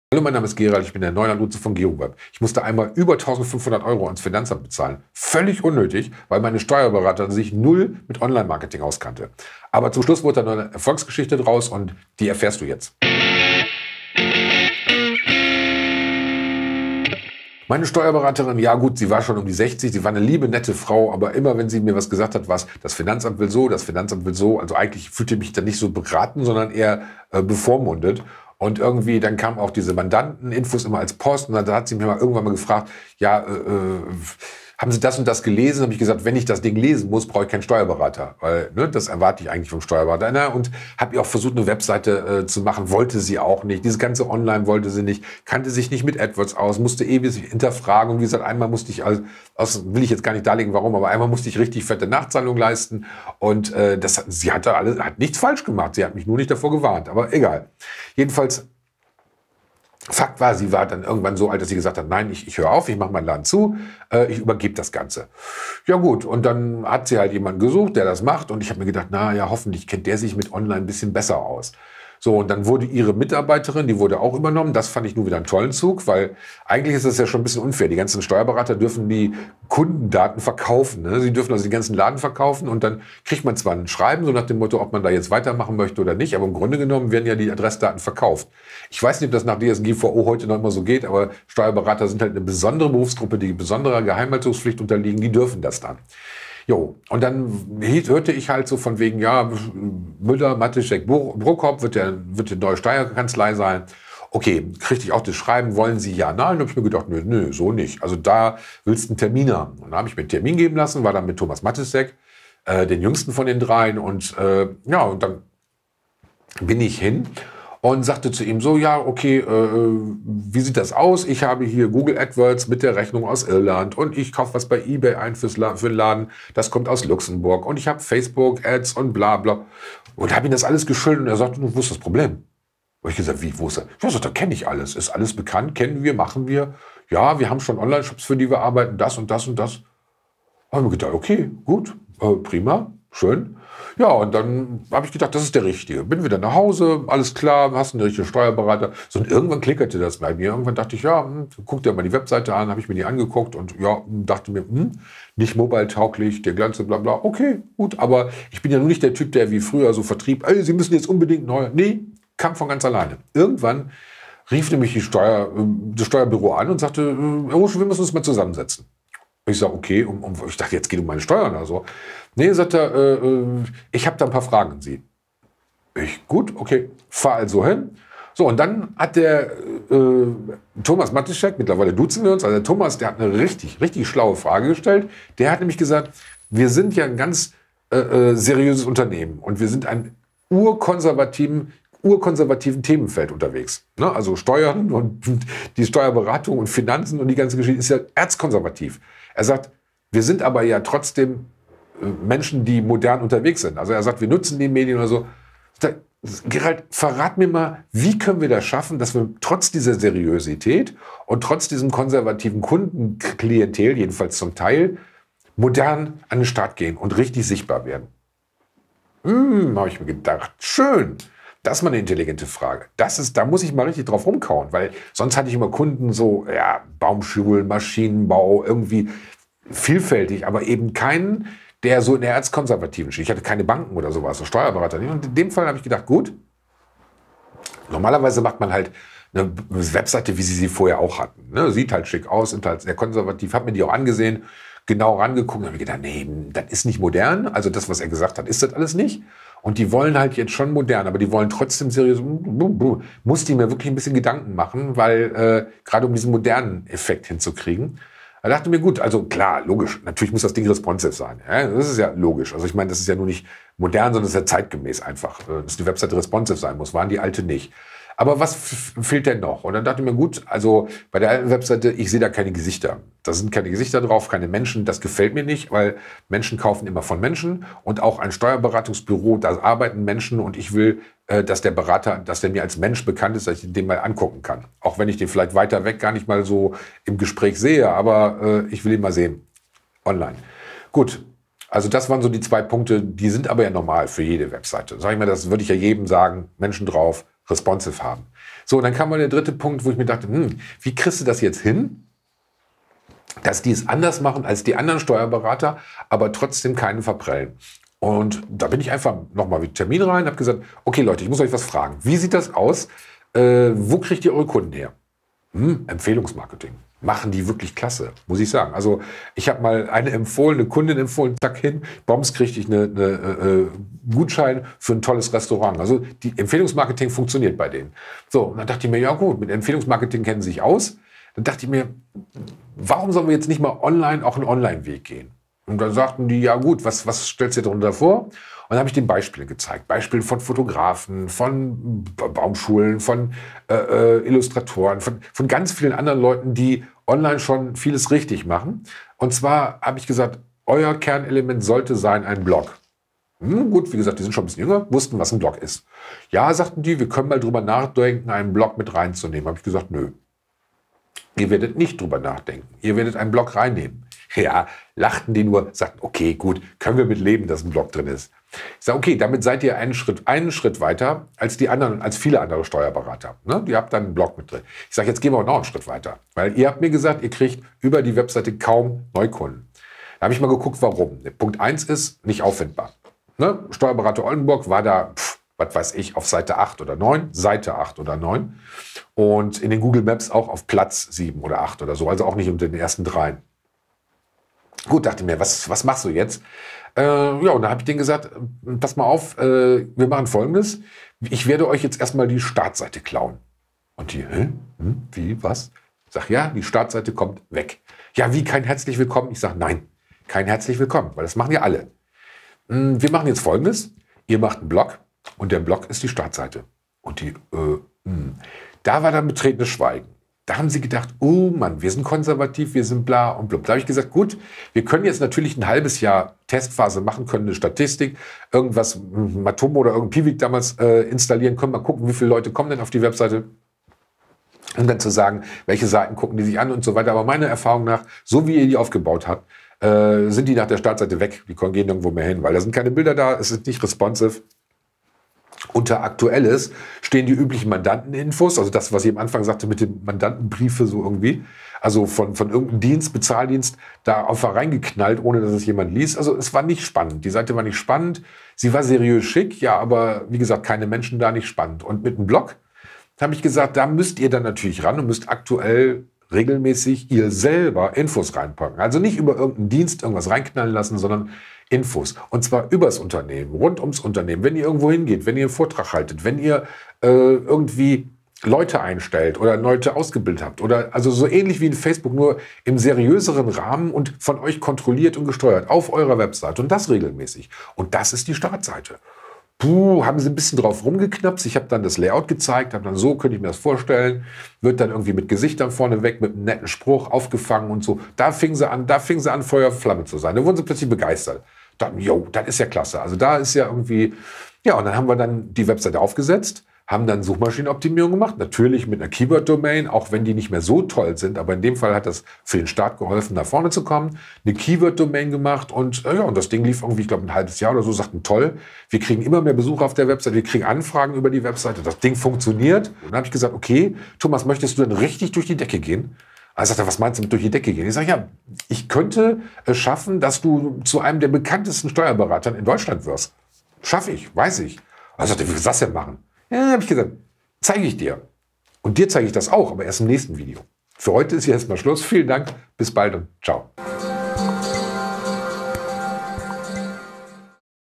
Digitalisierung - warum seriöse konservative Unternehmen online aktiver werden müssen
Digitalisierung ist für alle da. Auch seriöse konservative Unternehmen müssen online aktiver werden!

Was interessiert dich außer der DSGVO noch so an Internet für Unternehmen? Schreib es einfach in die Kommentare

Dieses Video wurde produziert mit:
Speed Booster